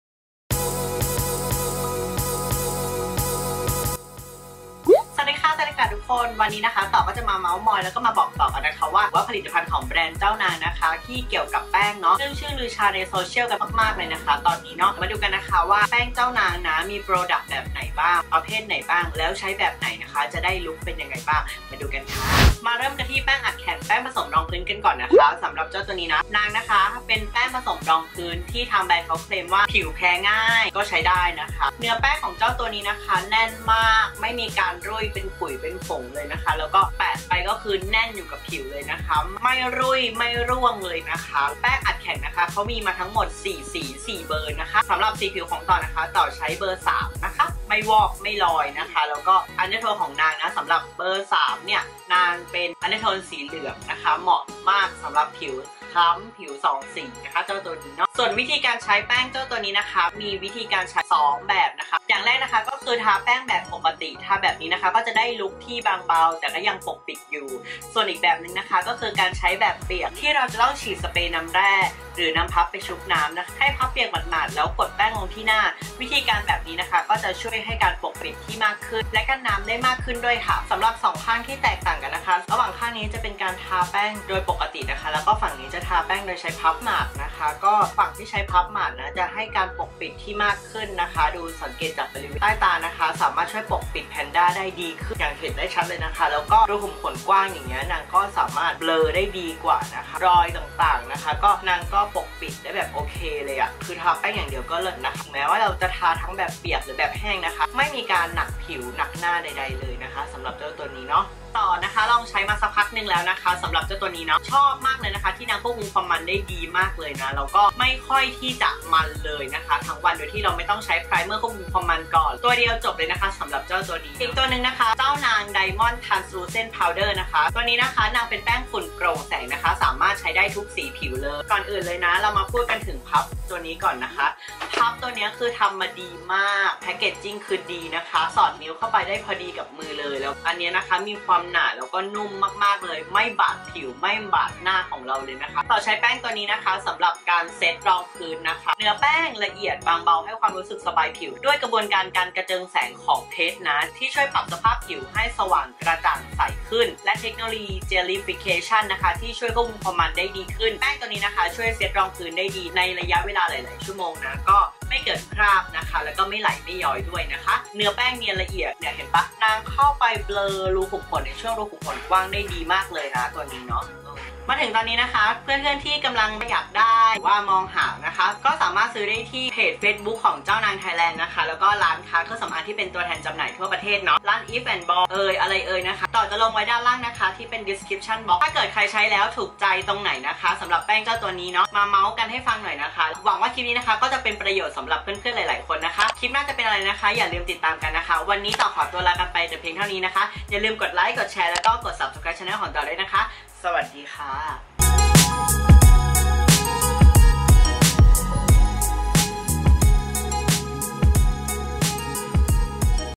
มาเม้าท์กันกับแป้งเจ้านางกันดีกว่า For เจ้านาง | KATOR ISME
ฮัลโหล คลิปนี้ต่อมาเม้าท์มอยบอกต่อกันกับแป้งเจ้านางนะคะ ว่าแป้งเขาที่โด่งดังเนี่ย อันเลื่องชื่อลือชาในโซเชียลเนี่ยมีแบบไหนบ้าง แล้วใช้แบบไหนจะได้เหมาะกับลุคไหนบ้าง มาดูกันค่ะ

Product in this vdo
แป้งเจ้านาง  2 way foundation : 399 บ. (โปรโมชั่น ซื้อสองตลับ 798 แถมฟรีแป้งฝุ่น 1 ตลับ)
แป้งเจ้านาง Diamond Translucent Powder : 319 บ. (โปรโมชั่น 230 บ.)

TOPS,EVEANDBOY,KONVY,BEAUTICOOL,LASHES,STARDUST,SUNCOSMATE,BEAUTY CLUB,BEAUTY MARKET,CJ EXPRESS และตัวแทนจำหน่ายทั่วประเทศ

My skin
สีผิว : ผิวแทน (สีรองพื้น revlon ฝาดำ No.330 Natural tan)
พื้นผิวหน้า : มีสิวบ้าง และ มีรอยดำจากสิวเยอะบริเวณช่วงหน้าผาก และ กราม
สภาพผิวหน้า : ผิวมัน มีรูขุมขนกว้างข้างจมูก